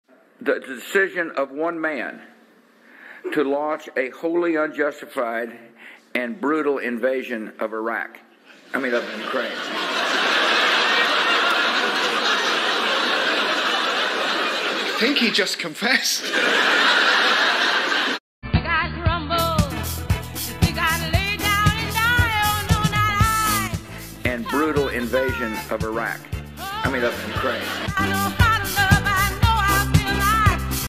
Iraq war
George W. Bush shoots himself in the foot criticising the Russian invasion of the Ukraine.